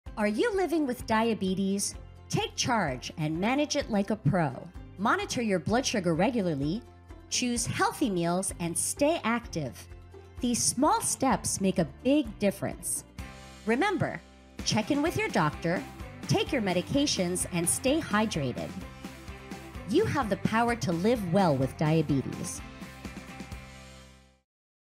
Live Well With Diabetes!
Are you living with diabetes? Take charge and manage it like a pro. Monitor your blood sugar regularly, choose healthy meals, and stay active. These small steps make a big difference.